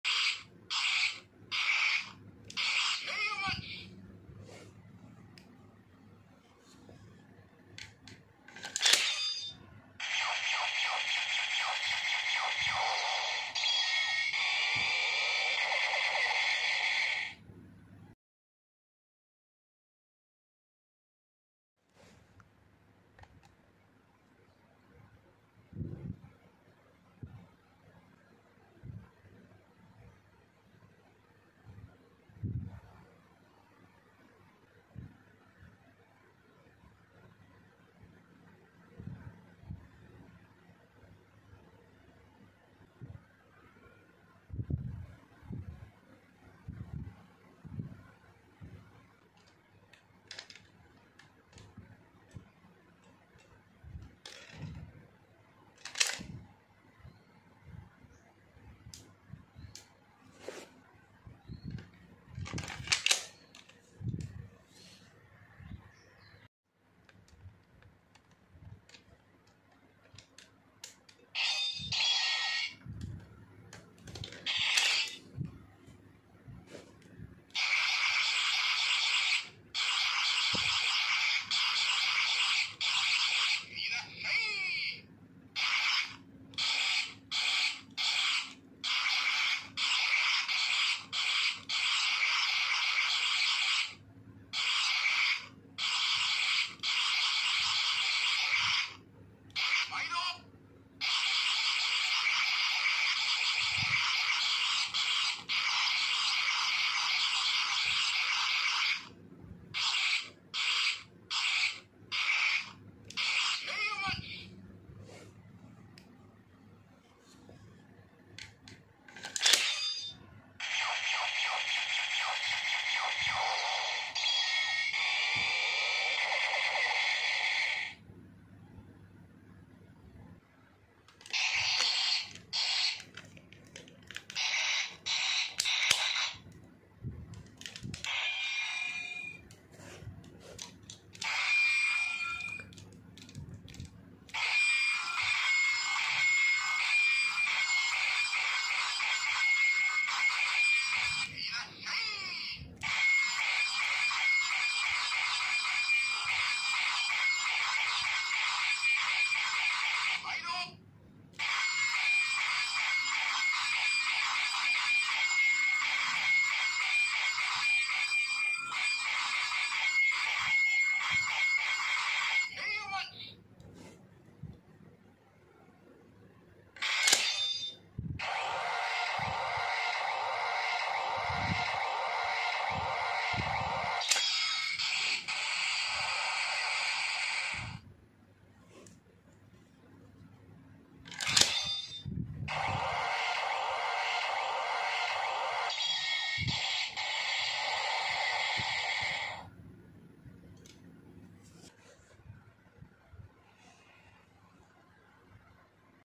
ได้เวลาทำซูชิแล้ว! รีวิว DX Sakanamaru Samurai Sentai Shinkenger (Review TH)
รีวิว DX ซาคานะมารุ
อาวุธ ดาบประจำตัวของ อุเมโมริ เก็นตะ
หรือ ชินเคนโกลด์  ที่สามารถเล่นกับ ฮิเด็นดิสก์ ได้
โดยลูกเล่นจะมีอะไรบ้างไปรับชมกันเลย

กลุ่ม Feacbook พูดคุยข่าวสารโทคุ